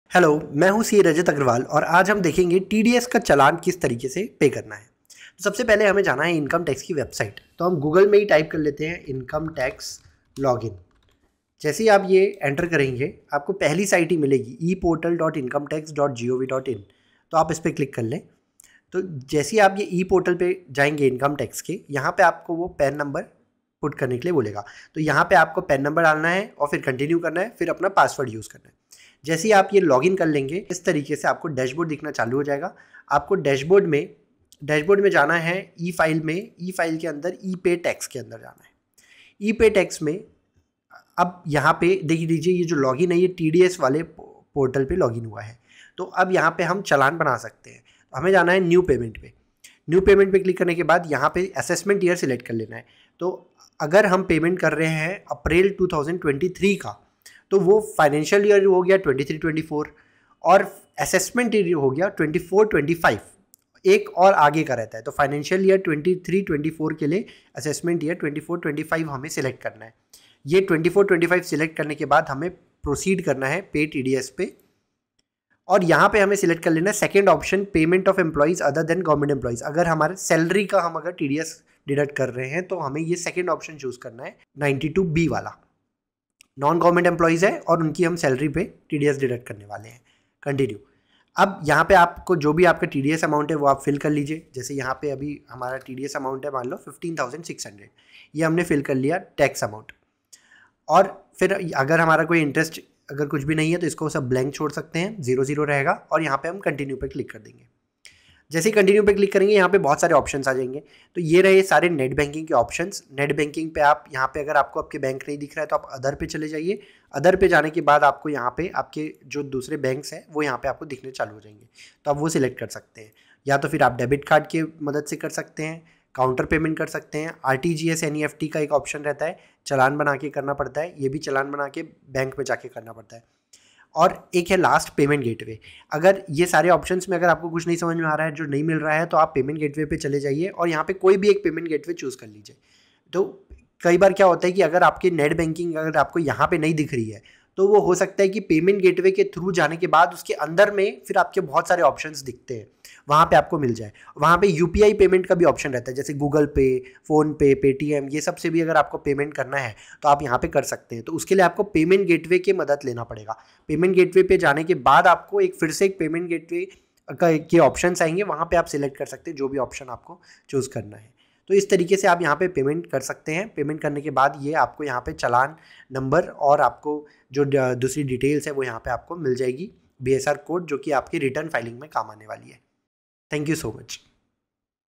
How to pay TDS Challan Online through Net-Banking , UPI etc
To pay TDS Challan online through the e-portal of the Income Tax Department, you can follow these steps:

Login to the e-Filing portal using your login credentials.

Once logged in, go to the "e-Pay Tax" option located under the "Quick Links" section.

Select "CHALLAN NO./ITNS 281" as the tax applicable.

Enter the required details such as TAN (Tax Deduction and Collection Account Number), Assessment Year, and other relevant details.

Verify the details entered and confirm the payment amount.

Choose your preferred payment method, such as net banking, debit card or credit card, and complete the payment process.

After the payment is made successfully, the system will generate a Challan Identification Number (CIN), which you should keep for future reference.

You can also generate a receipt of your TDS Challan payment by clicking on "View / Print Challan Details" and downloading the receipt.

Note: Make sure to verify the details entered before making the payment, and keep a record of the transaction for future reference.